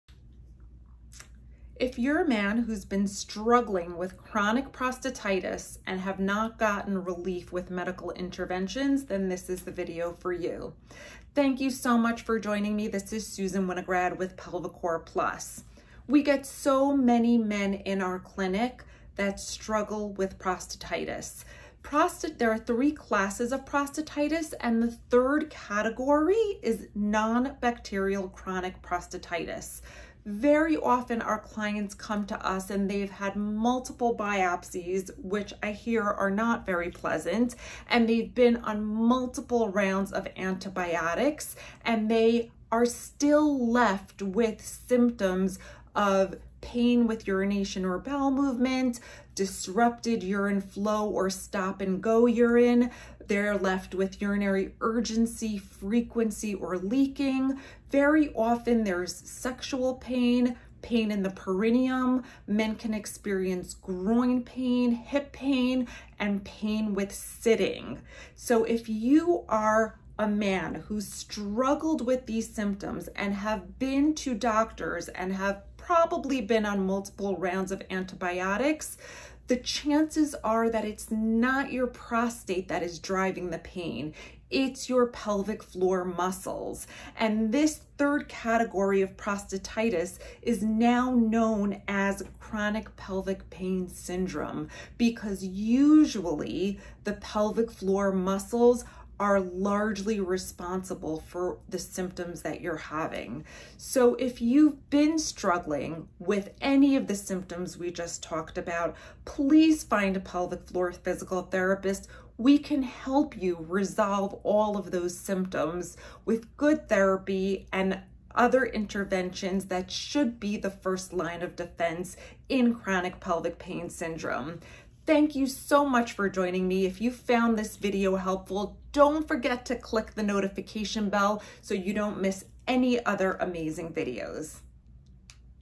Male Pelvic Pain: Prostatitis or Pelvic Floor; What's Causing the Pain???
✅ If you’re a man who’s been struggling with chronic prostatitis and have not gotten relief with medical interventions THEN this is the video for you!!!

✅ We help so many men in our clinic overcome their struggle with prostatitis.

✅ There are 4(I misspoke in the video here..) categories of prostatitis, the third category is called
 Non-Bacterial Chronic Prostatitis (CPPS)

✅ Very often our clients come to us having had multiple biopsies (which they unanimously report is NOT very pleasant) and they been on multiple rounds of antibiotics.

✅ They are still left with symptoms of pain with urination, interrupted urine flow, urinary urgency/ frequency or leaking. Pain with bowel movements and very often there’s sexual pain, pain in the perineum. Many men experience groin, hip pain and pain with sitting.

✅ So…. if you are A man who struggles with any of these symptoms, have been to doctors and have probably been on multiple rounds of antibiotics the chances are that it is NOT your prostate that is driving the symptoms, it’s your pelvic floor muscles.

✅ This third category of prostatitis is now known as Chronic Pelvic Pain Syndrome (CPPS) because usually the pelvic floor muscles are largely responsible for the symptoms.

✅ If this describes your situation and you still struggle with symptoms, please find a pelvic floor physical therapist. Pelvic floor PT should be the FIRST line of conservative treatment for CPPS. Healing is possible!!